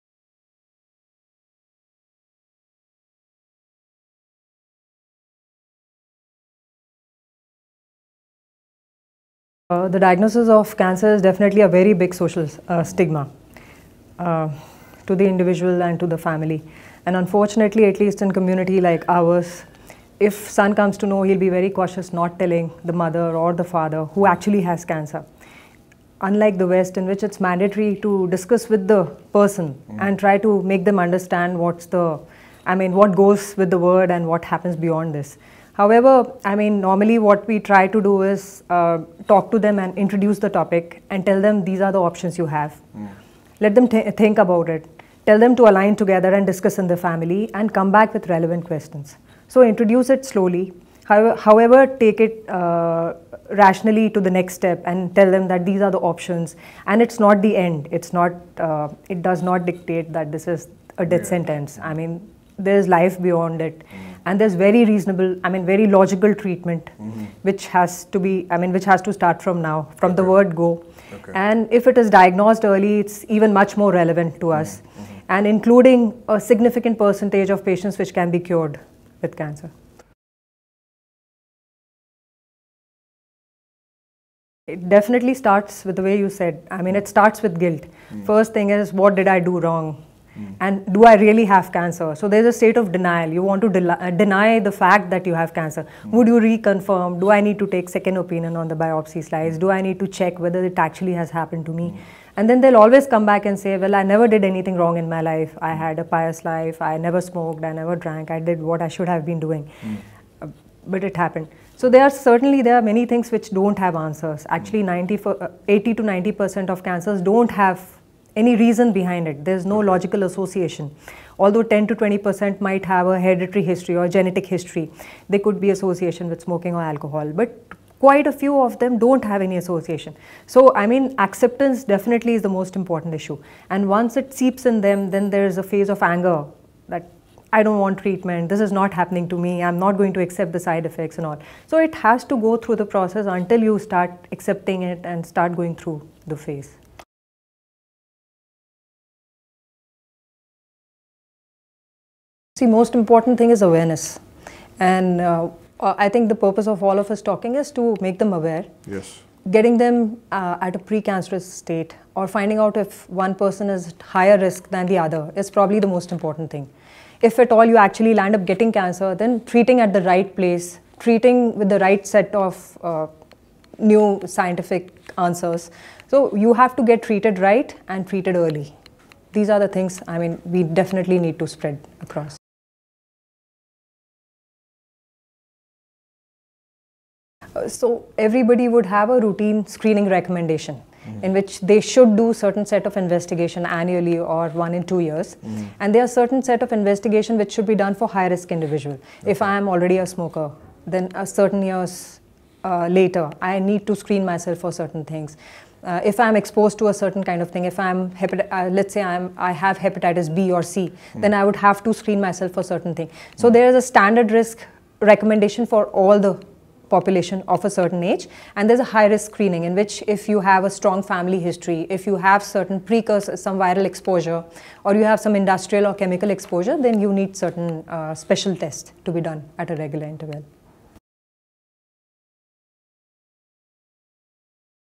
Dr Niti Raizada On Cancer
In conversation with Dr. Niti Raizada, Consultant Medical Oncologist and Hem-Oncologist about cancer and its effects on both the patients and their loved ones. She brought some relevant answers to the table that could be helpful for all.